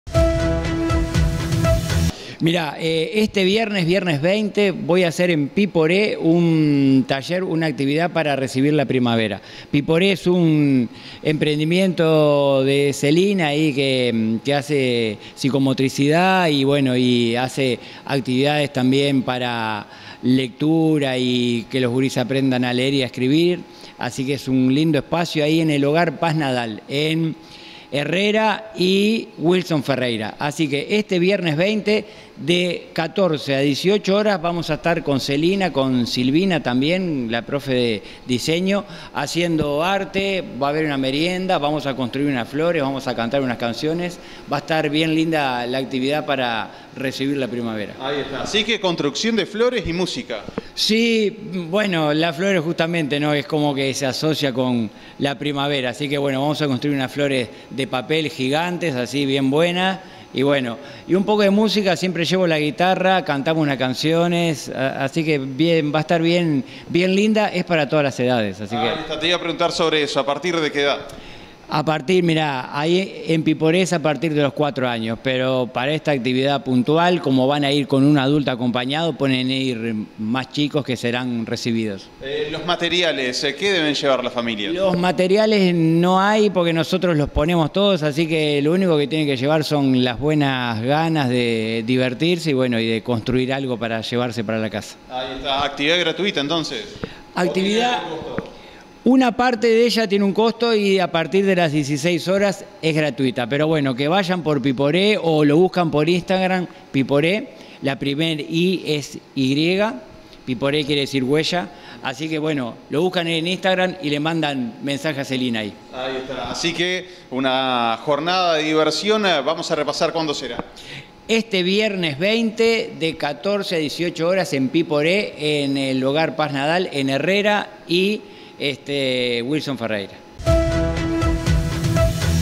En Pypare del Hogar Paz Nadal se desarrollará un taller de armado de flores
Será este viernes a las 14:00 horas.